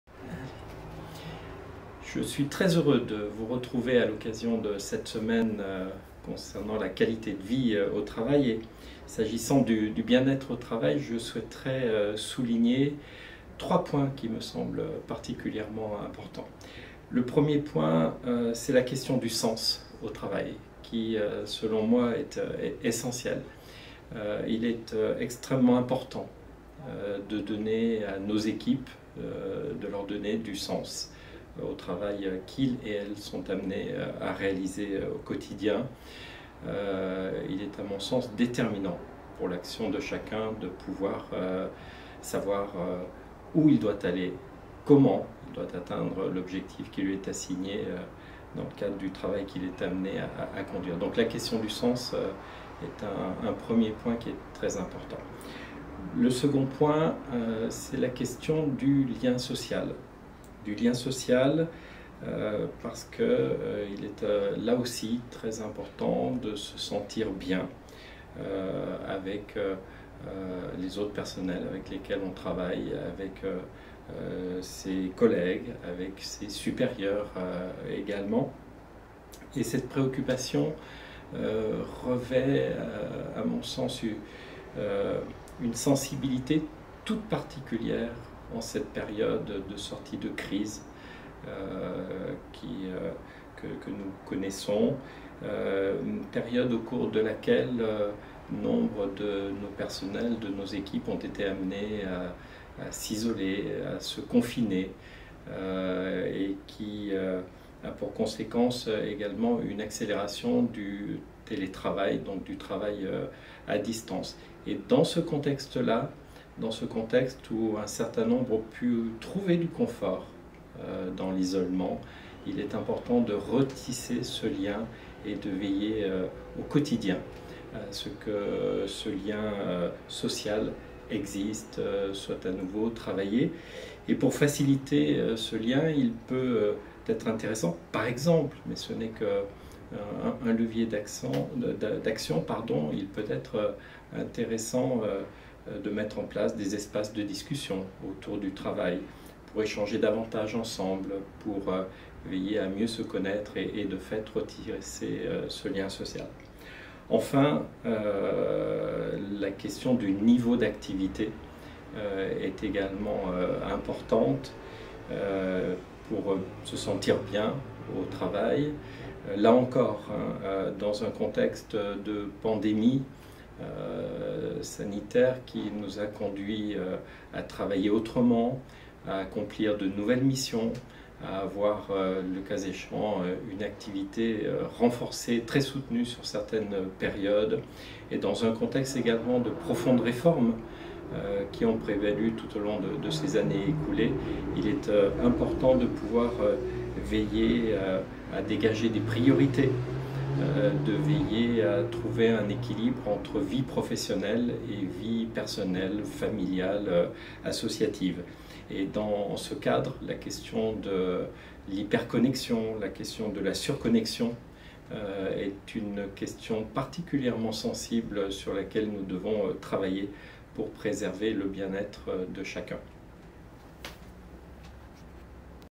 Semaine Qualité de Vie au Travail : Intervention du Secrétaire Général de l'académie d'Aix-Marseille
Monsieur le Secrétaire Général de l'académie d'Aix-Marseille présente et introduit la semaine de la Qualité de Vie au Travail qui se déroulera du lundi 20 juin 2022 au vendredi 24 juin 2022 sur le thème "En quête de sens au travail". Ce thème semble s’imposer comme une évidence : après plus de deux ans de crise, on n’a jamais eu autant besoin de trouver du sens à notre travail et de se sentir utile. La réalisation de cette semaine s'est effectuée en collaboration avec l'Agence Nationale de l'Amélioration des Conditions de Travail.